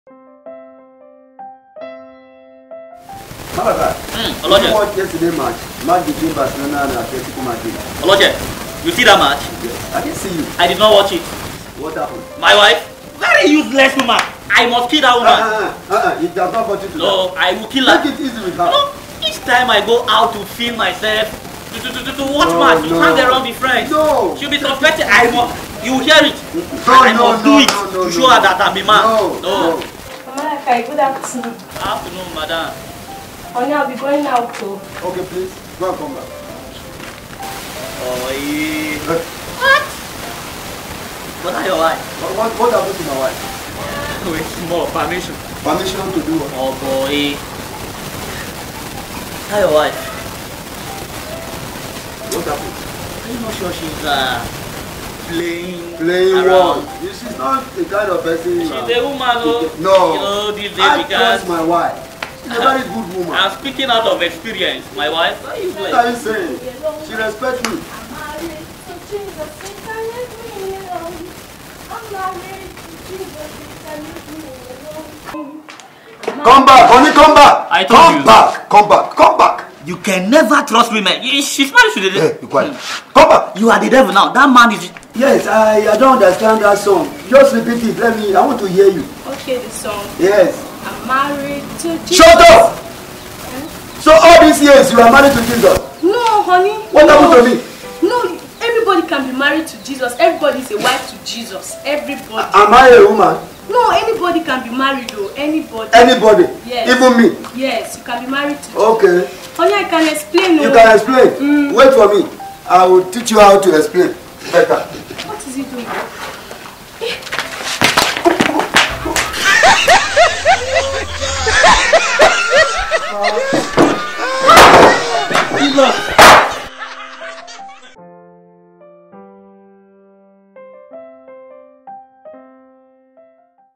Married to Jesus Comedy (Bow laugh & learn YouTube comedy)
Married to Jesus comedy is a Very funny comedy from Barn of Wisdom crew.
Malachi causes a major crises between Oloche and his wife.
Don't laugh alone.